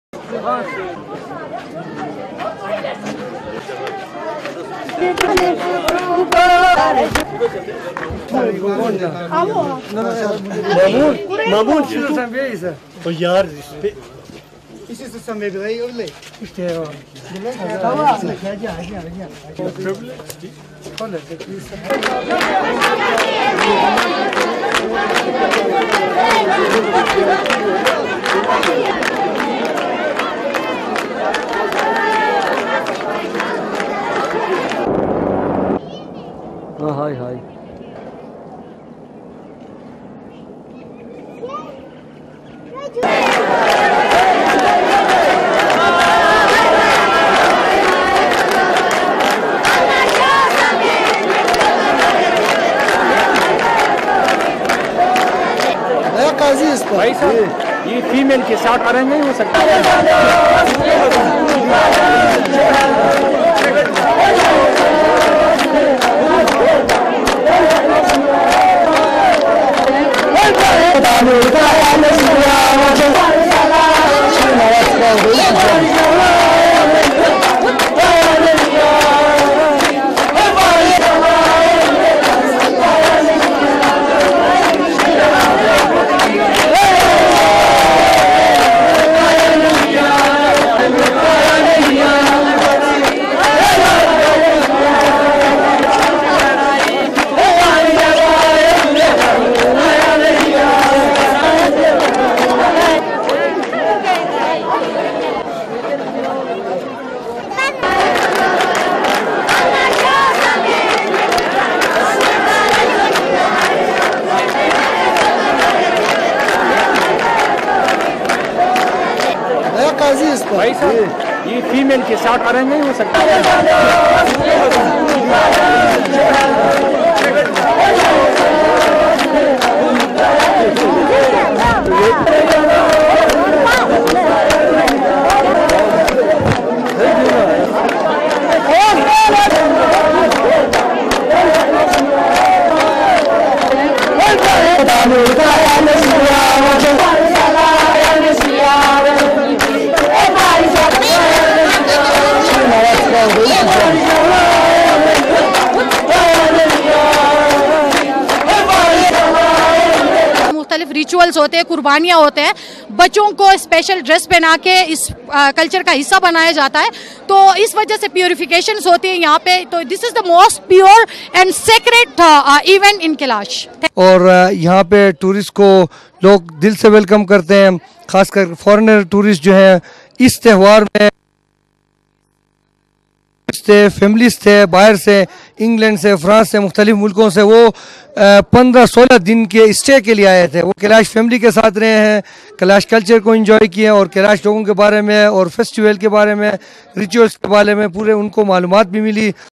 Kalash winter festival 2018.
Eight couples tied the knot on the concluding day of Kalash winter festival on Dec 22, 2018.